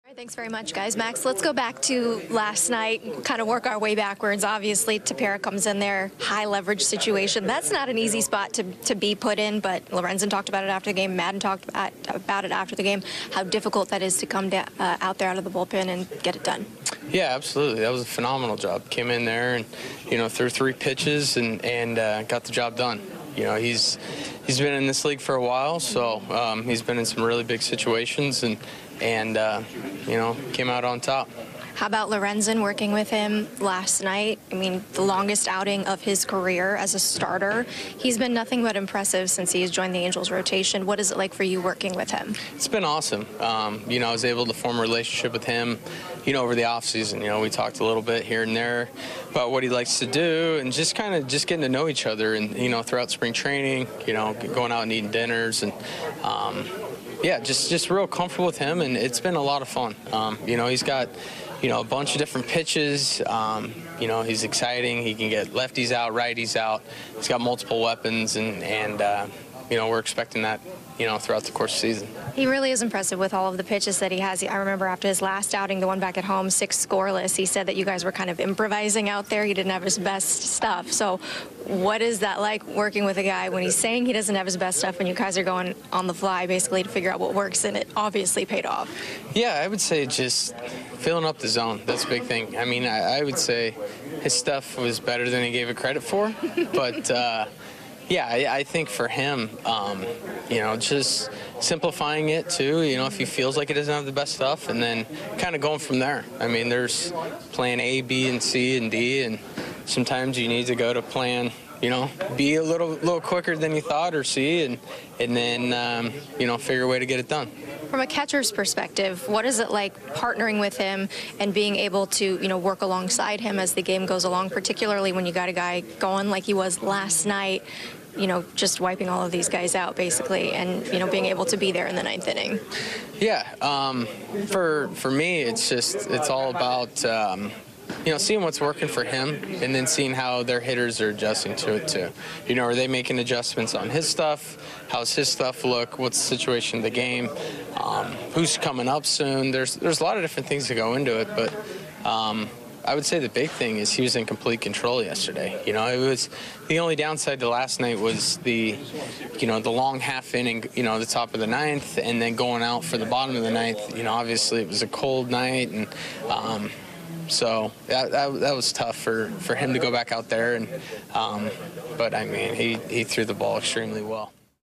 【現地の反応】【日本語訳】エンジェルスの正捕手、マックス・スタッシのインタビュー
5月2日の試合より、マックス・スタッシ捕手の試合前インタビューを和訳してお届けします。隠れイケメン？！